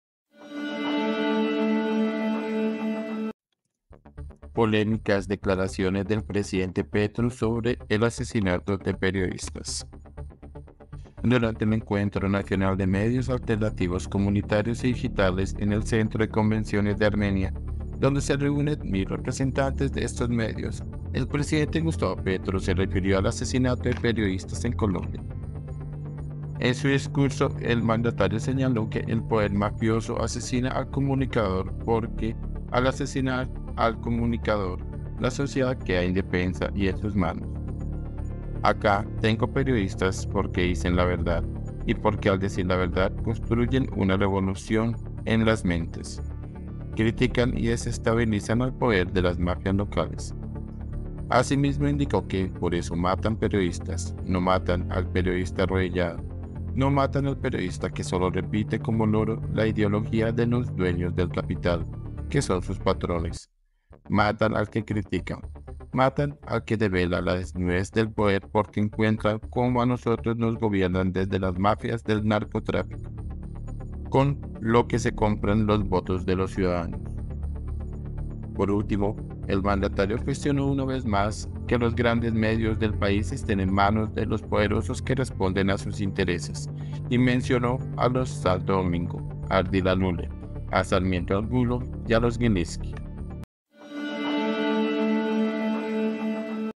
❌ POLÉMICAS DECLARACIONES DEL PRESIDENTE PETRO SOBRE EL ASESINATO DE PERIODISTAS
Durante el Encuentro Nacional de Medios Alternativos, Comunitarios y Digitales en el centro de convenciones de Armenia, donde se reúnen 1.000 representantes de estos medios, el presidente Gustavo Petro se refirió al asesinato de periodistas en Colombia.
W RADIO